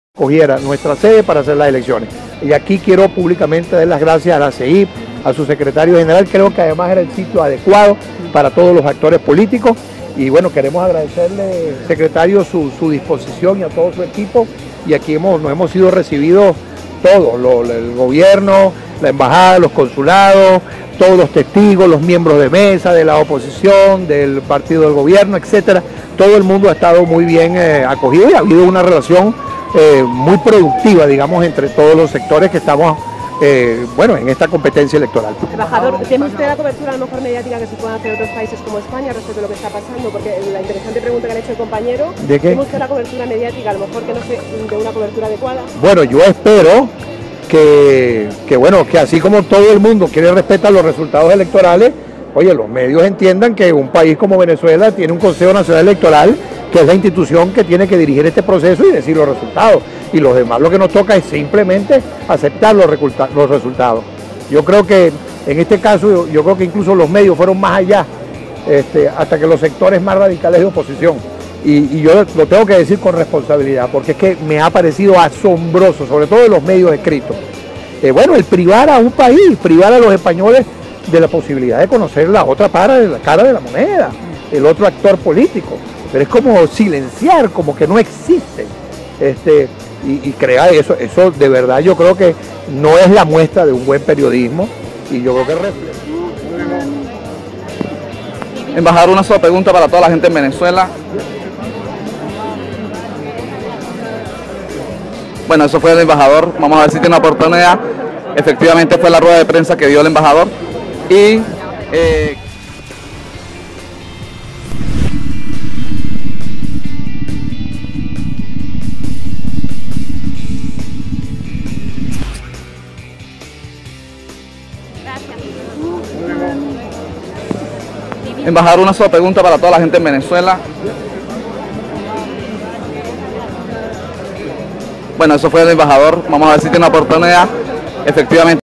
Enbajador De Venezuela Se Queja De Los Medios pero luego no les contesta
Chevere quejarse